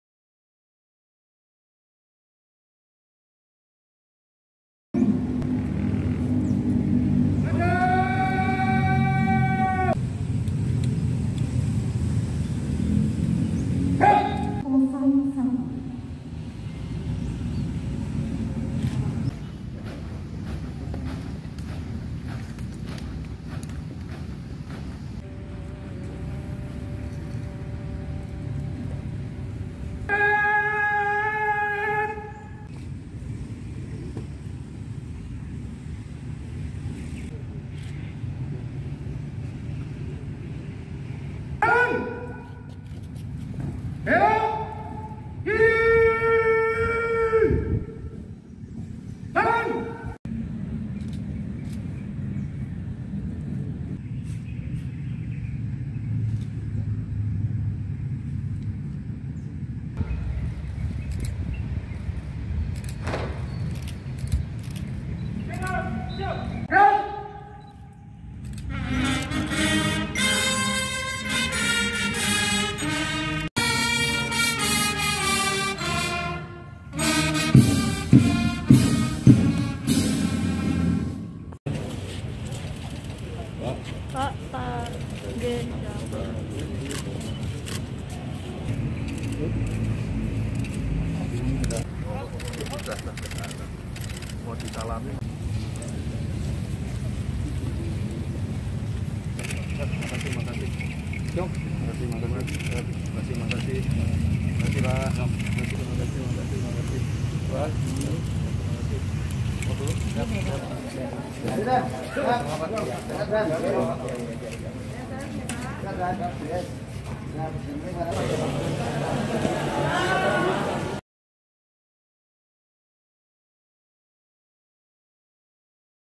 HUT RI ke 76- Gibran Pimpin Upacara Gunakan Baju Beskap Langenharjan.
Peringatan HUT RI ke 76 tahun di kota solo

Walikota Solo Gibran gunakan baju adat beskap Langenharjan sedangkan Wakil Walikota solo Teguh Gunakan baju adat Nusa tenggara timur